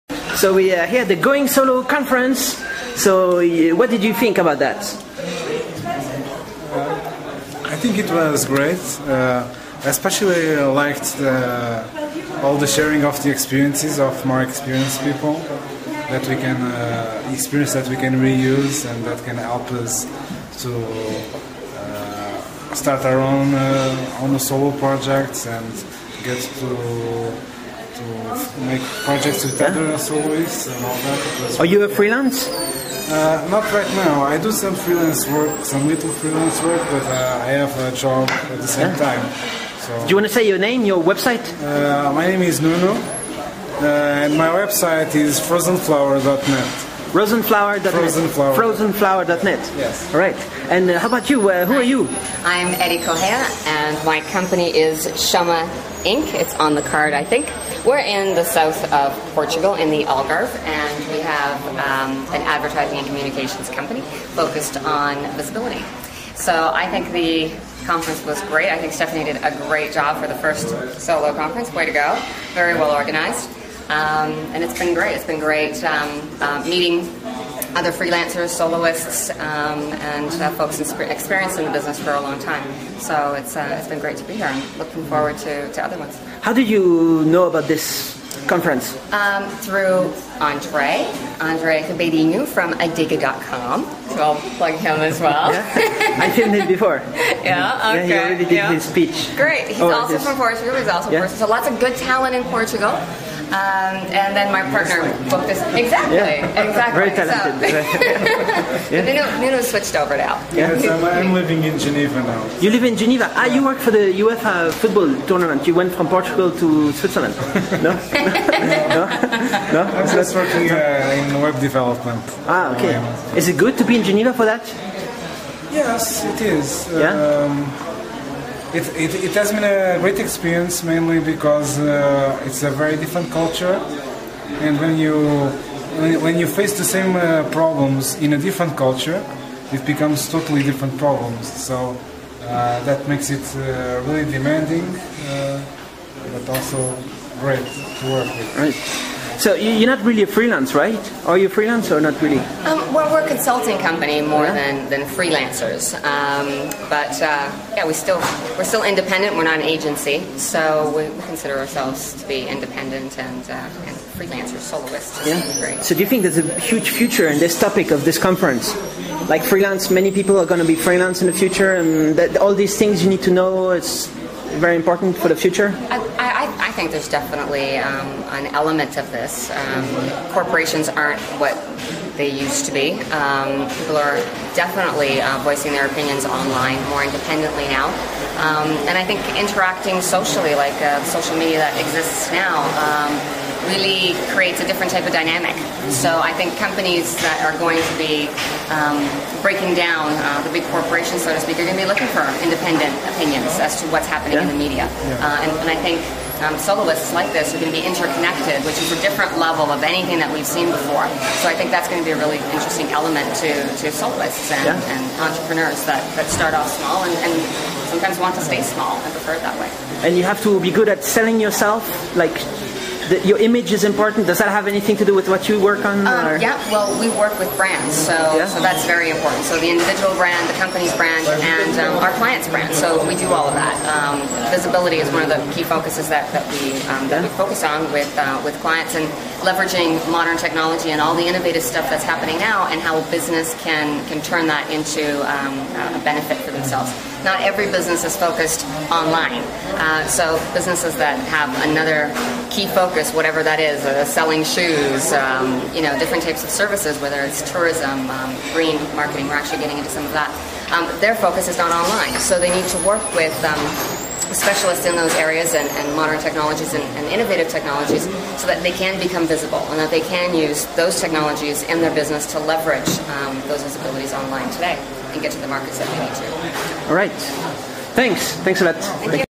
Nuno Barreto and Eddie Correia at Going-Solo Lausanne 2008
is a one-day educational conference for freelancers and small business owners of the internet industry and beyond.

It was organized by Going Far, Stephanie Booth’s new media company, and took place in Lausanne, on the shores of Lake Geneva in Switzerland, on May 16th, 2008.